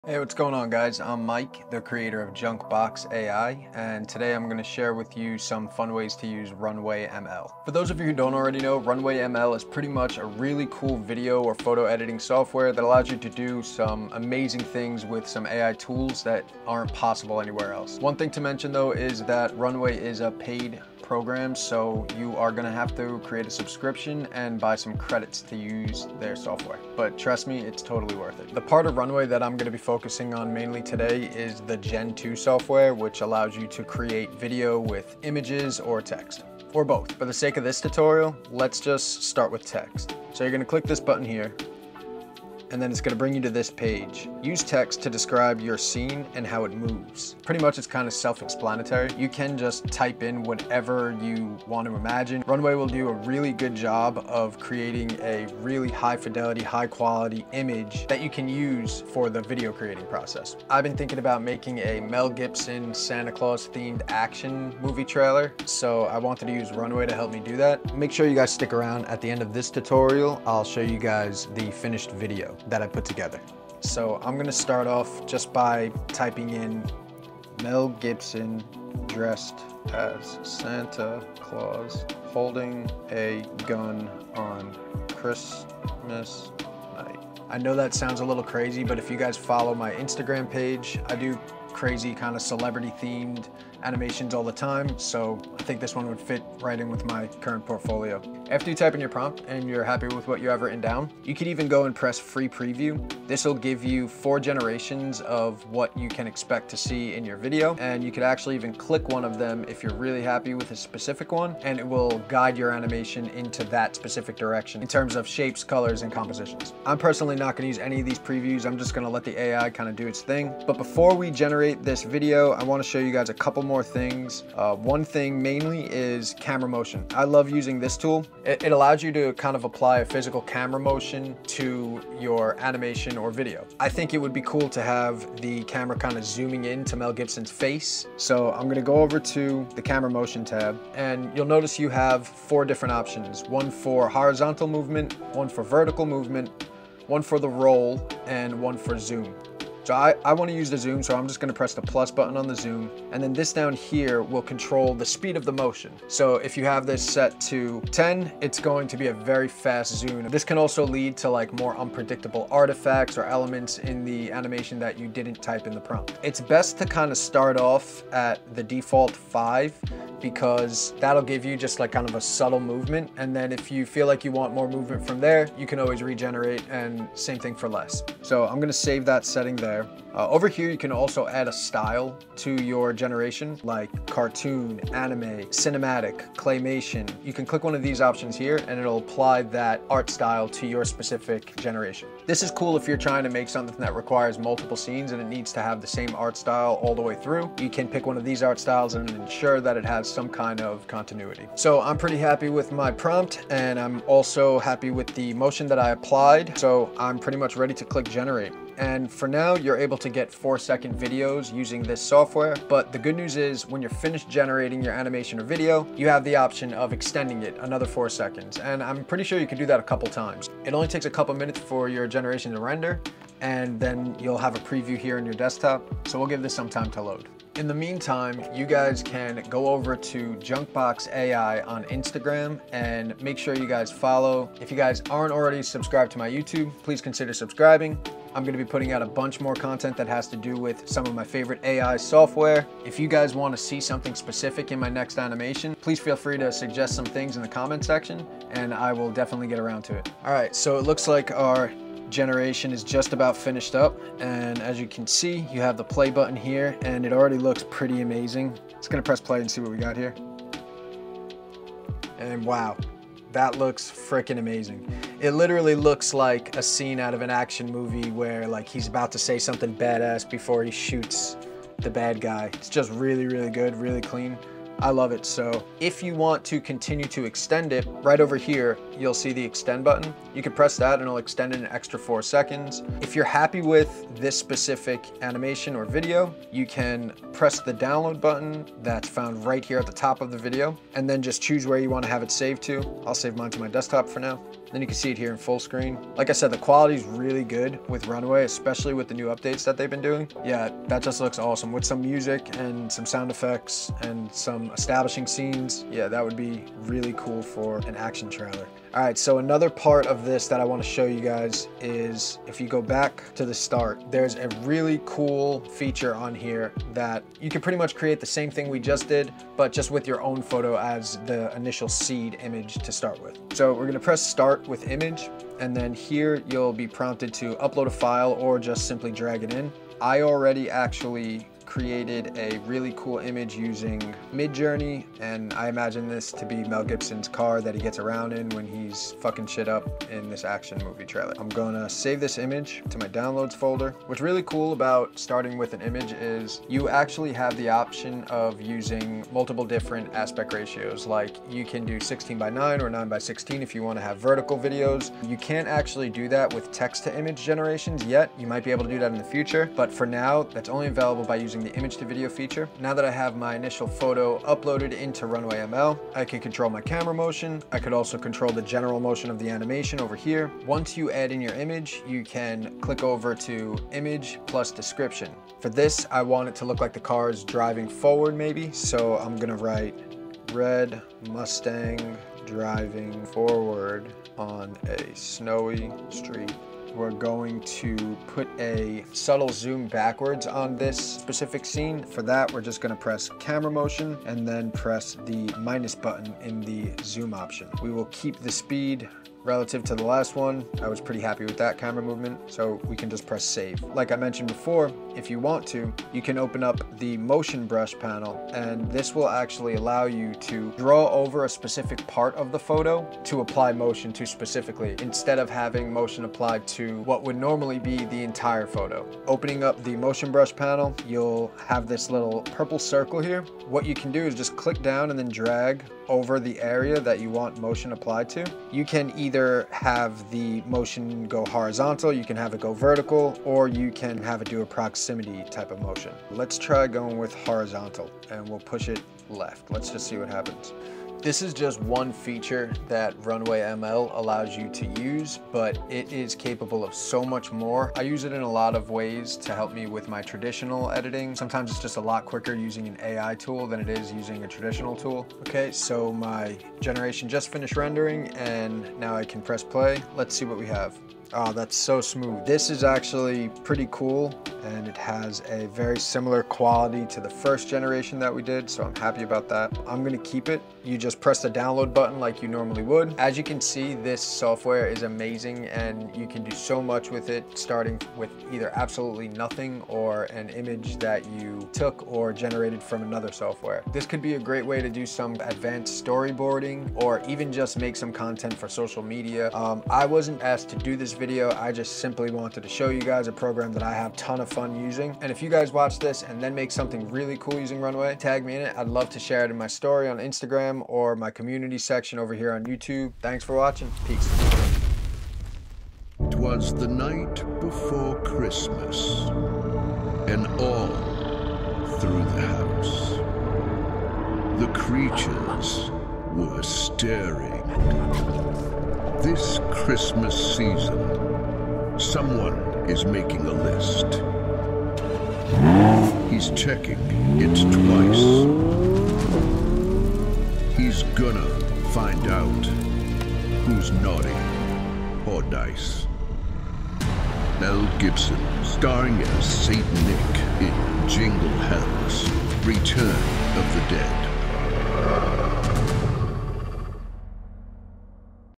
Making an ACTION MOVIE TRAILER using RunwayML Gen2
Runway ML's Gen-2 is available to everyone! This is a guide to my process in creating an action movie trailer using A.I. We discuss Text to Video, and Image to video modes, we also go over camera motion and other tools.

The full Trailer for "Jingle Hells Return of the Dead" is featured at the end of this tutorial so stick around to check it out!